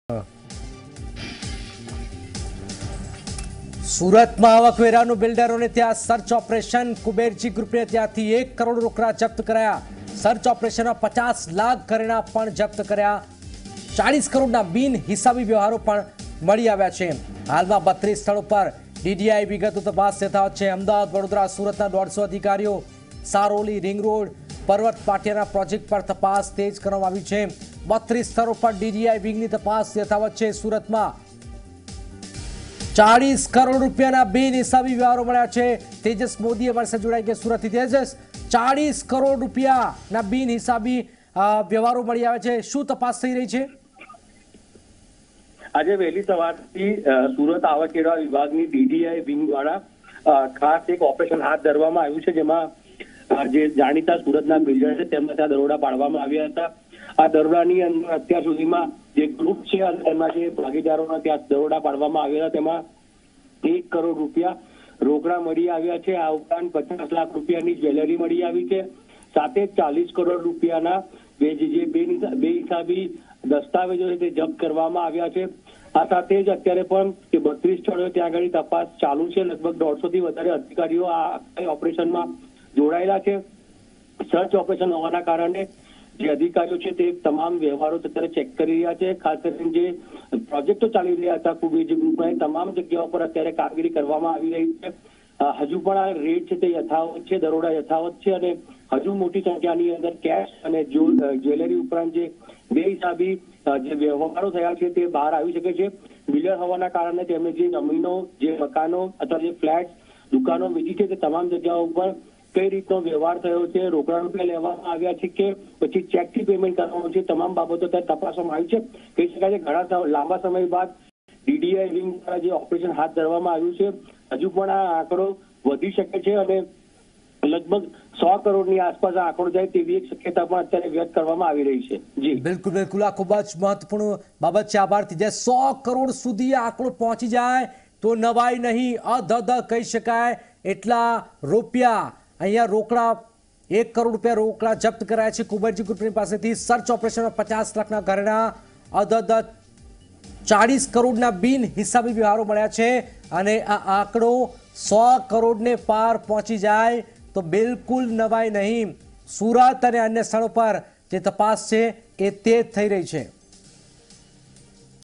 Search Operation Of IT To Builders Of Surat
સુરતના બિલ્ડરોને ત્યાં આઇટીનું સર્ચ ઓપરેશન

સુરતમાં આવકવેરા વિભાગ દ્વારા બિલ્ડરોને ત્યાં સર્ચ ઓપરેશન હાથ ધરવામાં આવ્યું હતું. કુબેરજી ગ્રુપને ત્યાંથી 1 કરોડ રોકડા જપ્ત કરવામાં આવ્યા હતા. સર્ચ ઓપરેશનમાં 50 લાખ ઘરેણાં પણ જપ્ત કર્યા હતા. 40 કરોડના બિન હિસાબી વ્યવહારો મળી આવ્યા હતા. હાલમાં 32 સ્થળો પર DDI વિંગની તપાસ યથાવત છે. અમદાવાદ, વડોદરા અને સુરતના 150 અધિકારીઓ તેમજ સારોલી, રિંગરોડ, પર્વતપાટિયાના પ્રોજેક્ટો પર તપાસ હાથ ધરવામાં આવી હતી.